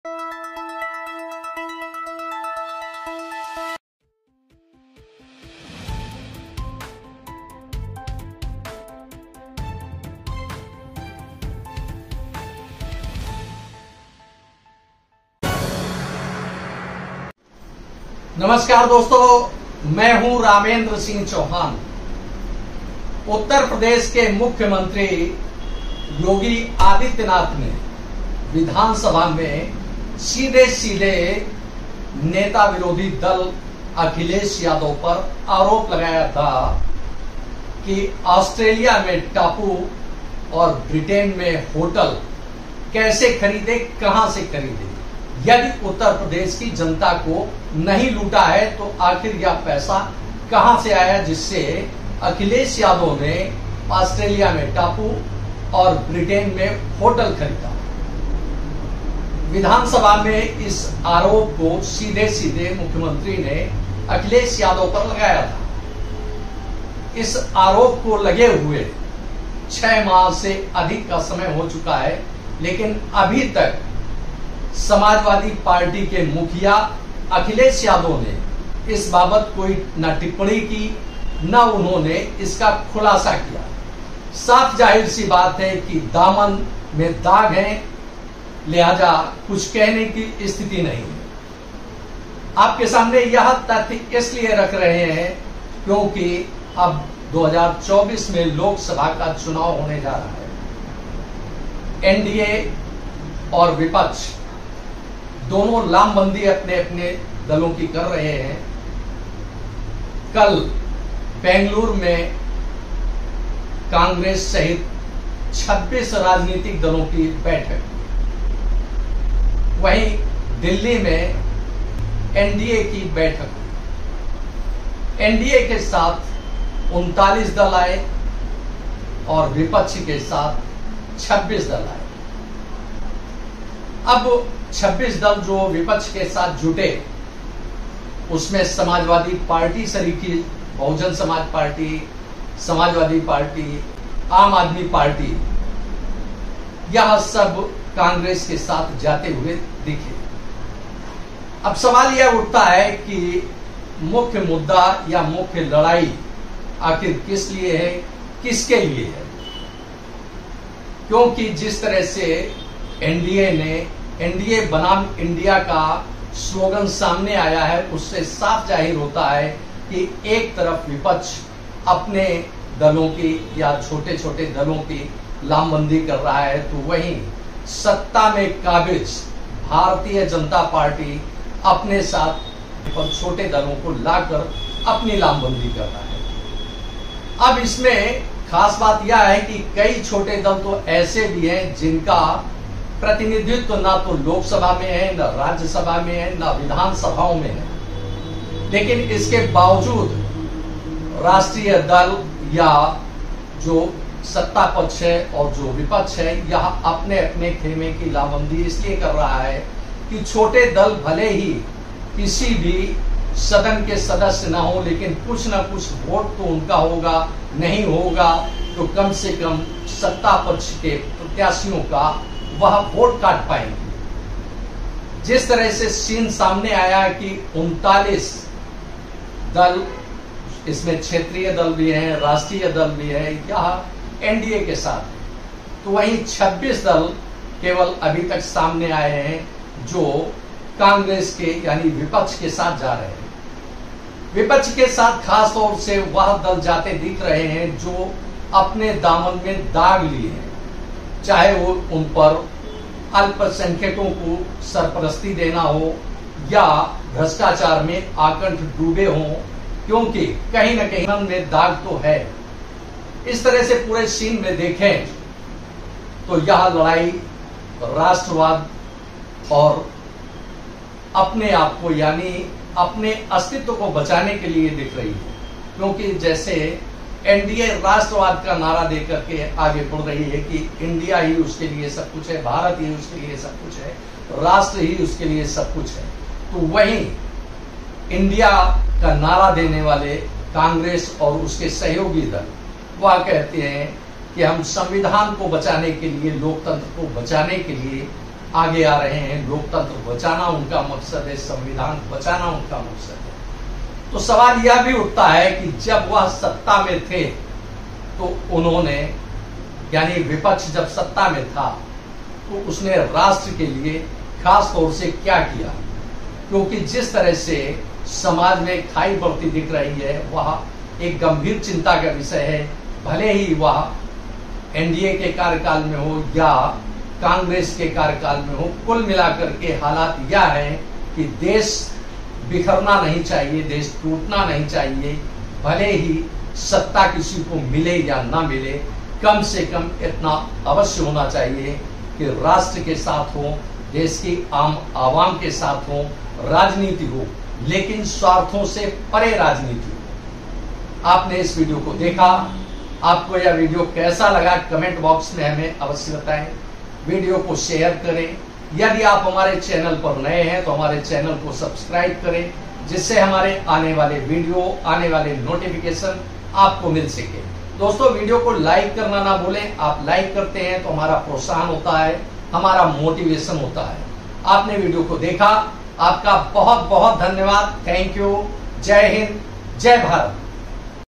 2024 लोकसभा चुनाव की बड़ी चुनौती छोटे दलों को साधने की कोशिश BJP CONG POLITICS RAMENDRA SINGH CHAUHAN
2024 लोकसभा चुनाव की बड़ी चुनौती
छोटे दलों को
साधने की
कोशिश.